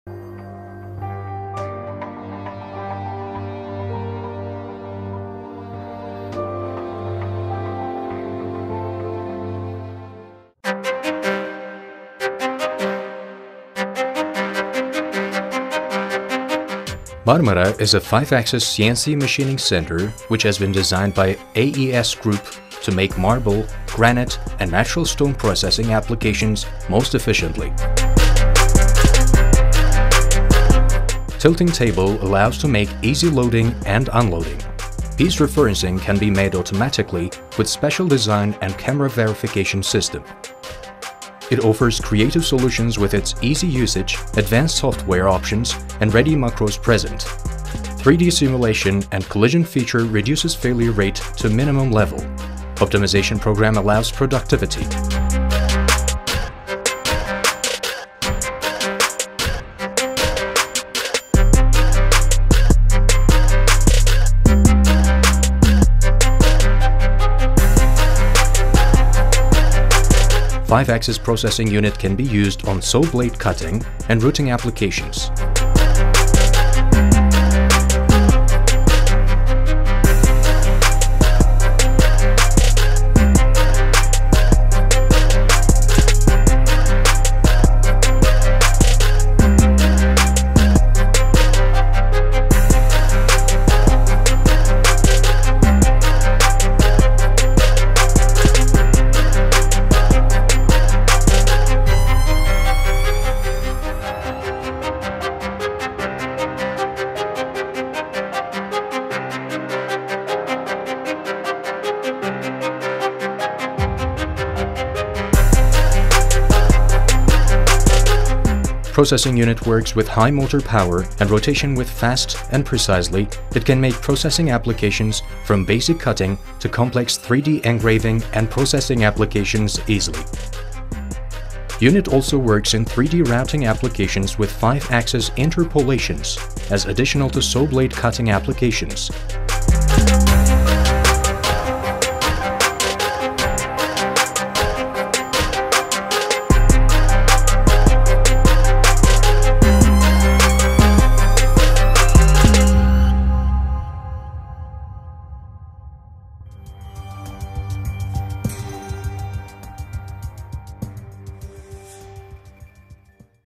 MARMARA - 5 Axis CNC Machining Center / Unique Solution for Natural Stone Processing
MARMARA is a CNC Machining Center that has 5 axis interpolation feature. It presents most suitable solutions for your needs with high quality components, strong body structure and special design ensurer working without any problem even most hardest working conditions.

MARMARA has been designed for marble, granite and natural stone processing applications most efficent way. Machine can make 5 axis routing applications easily besides saw cutting applications.